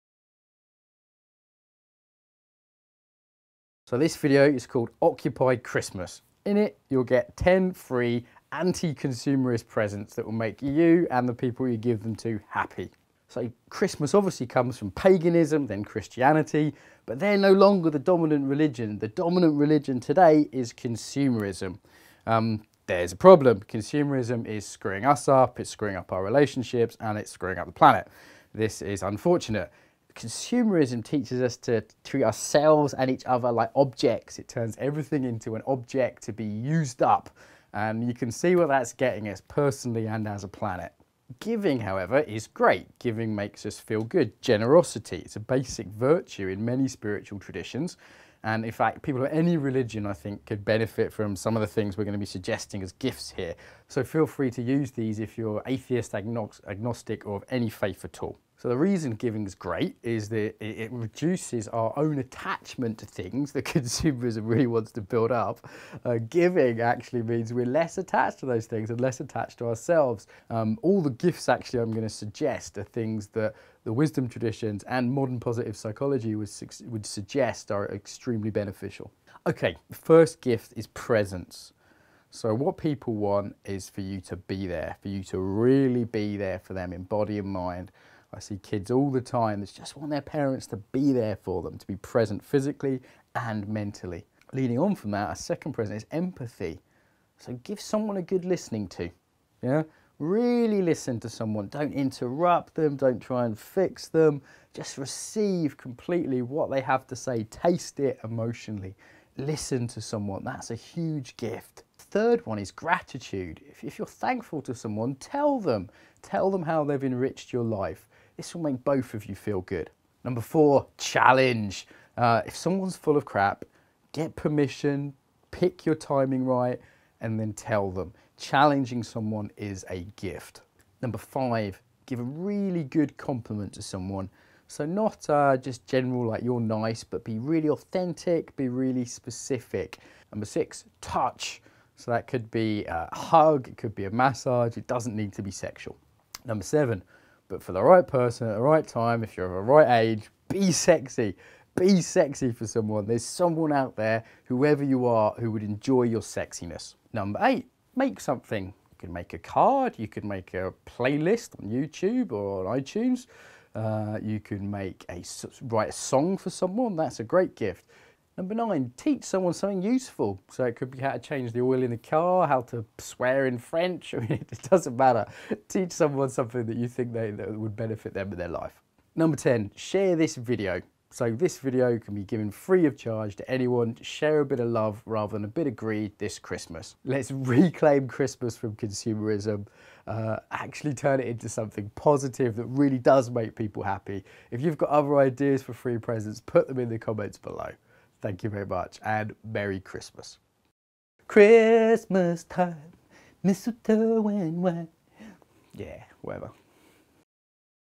10 Free Non-Consumer Gifts for Happiness - Occupy X-Mas!
Occupy X-Mas - 10 Free non-consumer gifts/ presents to make people happy - scientifically proven from positive psychology - with Mark Walsh from Integration Training

Ten free X-mas Gifts - Presents:
- Presence
- Empathy
- Gratitude
- Challenge
- Compliments
- Touch
- Be sexy!
- Make something
- Teach something
- Share this
Image - Banksy - used without permission :-)